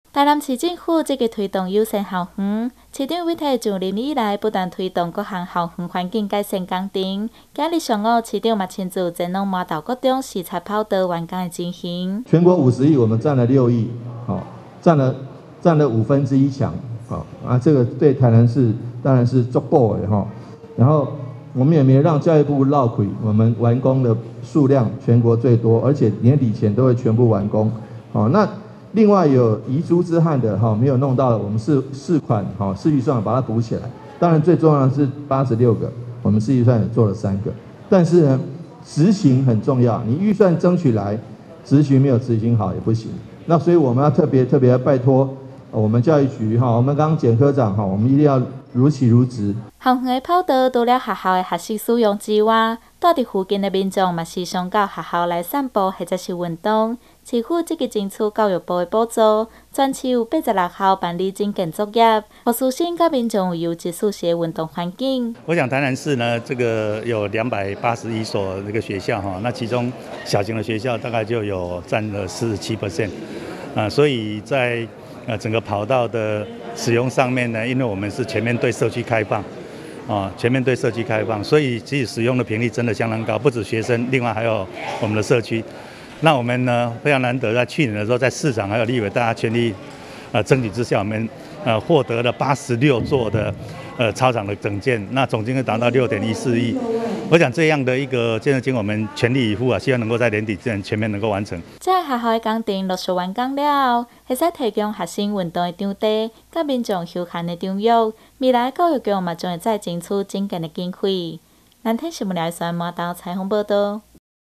改善老舊操場跑道 市長視察麻豆國中成果20220810
台南市有許多學校的操場都很老舊，學生在跑跳時有很大的機率會摔傷，為了給學生安心又安全的運動環境，在教育部及市府教育局的支持下，本市現在有80餘所學校，辦理操場跑道改善作業，今(10)日一早市長黃偉哲也前往麻豆國中視察操場及周邊設施整建情形，希望給學生一個安全的運動場地。